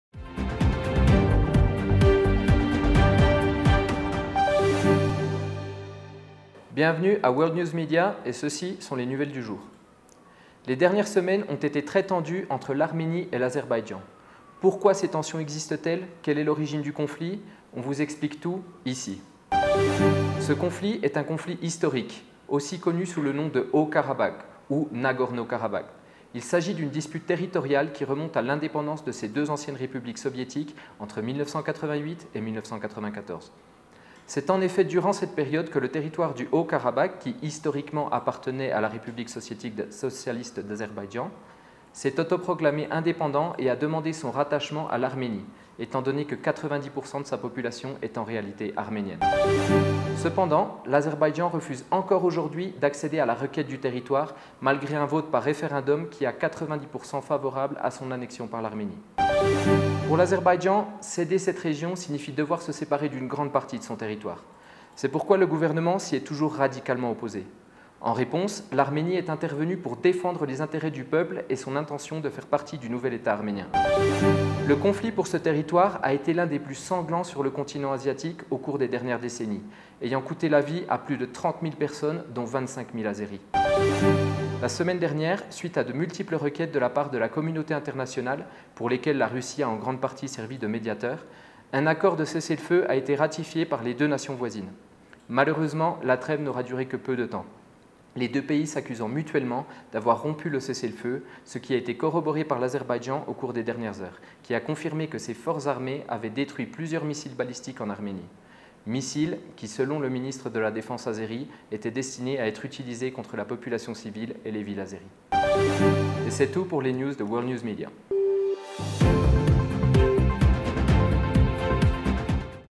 Watch Sky World News Media
Report from France: What role are Russia and Turkey playing in the Nagorno-Karabakh conflict?